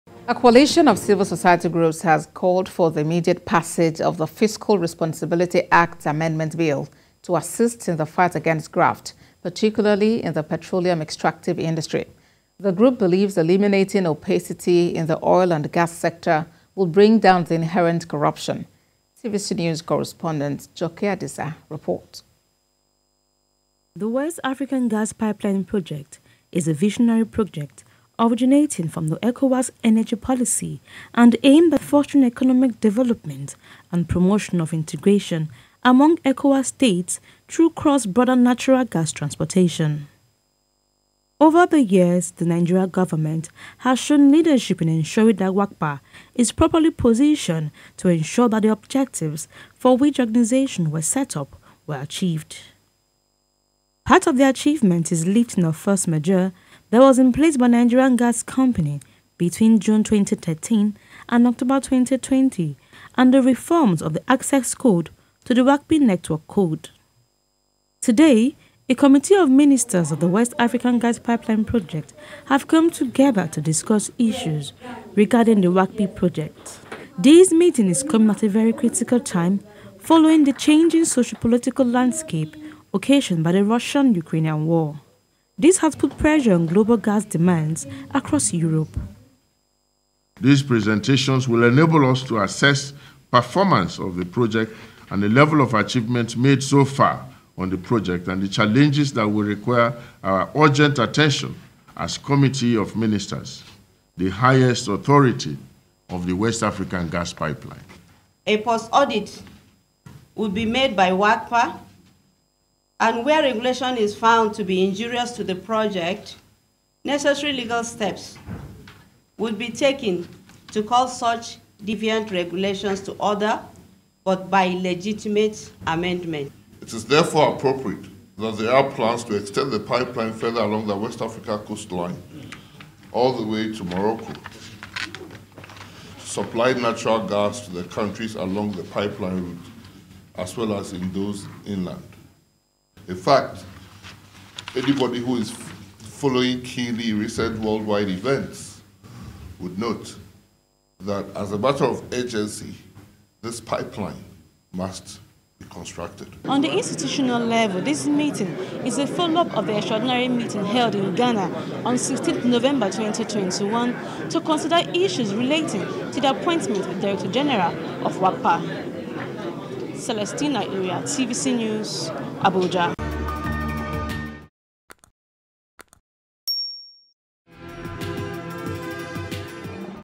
WAPGA Ministers Meet To Strengthen Cooperation
A coalition of civil society groups has called for the immediate passage of the Fiscal Responsibility Act Amendment Bill to assist in the fight against graft, particularly in the Petroleum Extractive Industry.
The group believes eliminating opacity in the oil and gas sector will bring down the inherent corruption